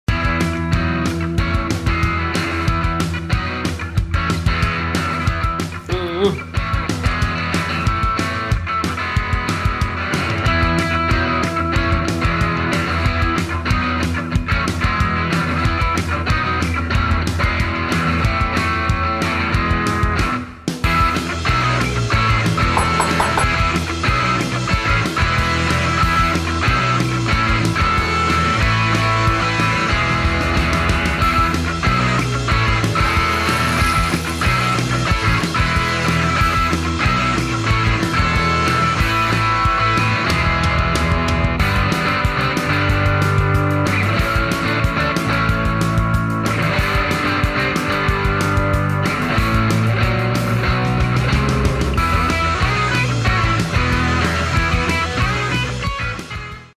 FALAFEL: Vegetarian Bites from Refika’s Kitchen no:1
Today I have the best falafel recipe for you! You just need some small tricks to make this crispy, delicious gluten free and vegan miracle. We made a delicious plate with the hummus and spoon salad as supporting actors, also not forgetting the deliciously crunchy simit! 😍